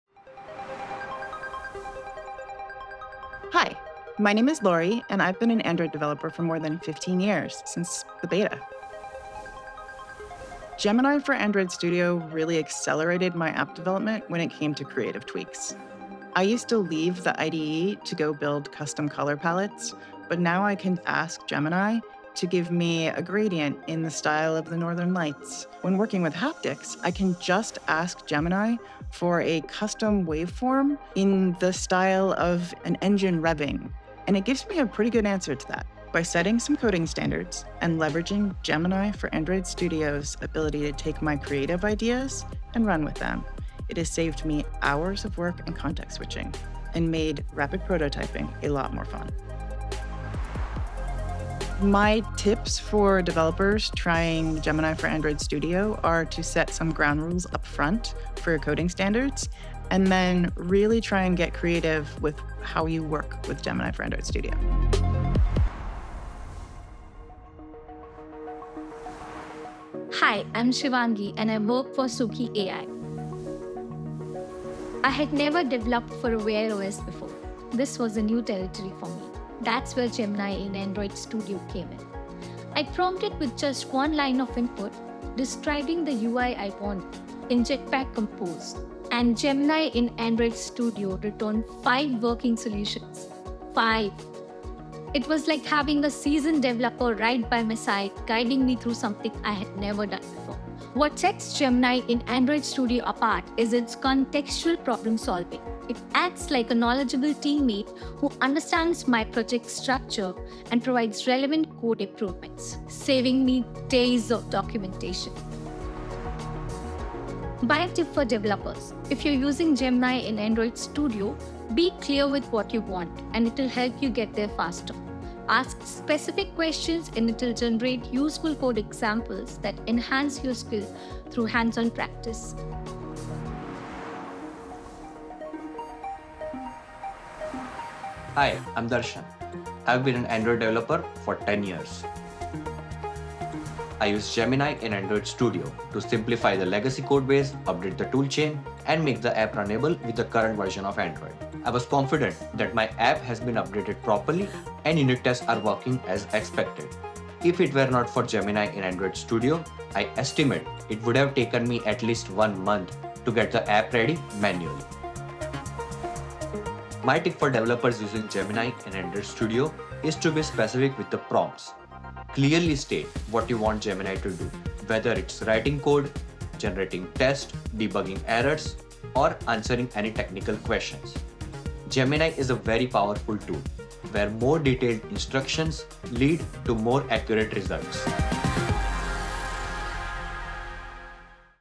Learn how Android devs are getting the most out of Gemini in Android Studio
We're excited to see Android developers sharing some of their best tips and tricks to get the most value out of Gemini in Android Studio.

Gemini in Android Studio just got a huge update, making it useful across every stage of the dev lifecycle.

Products Mentioned: Gemini, Android Studio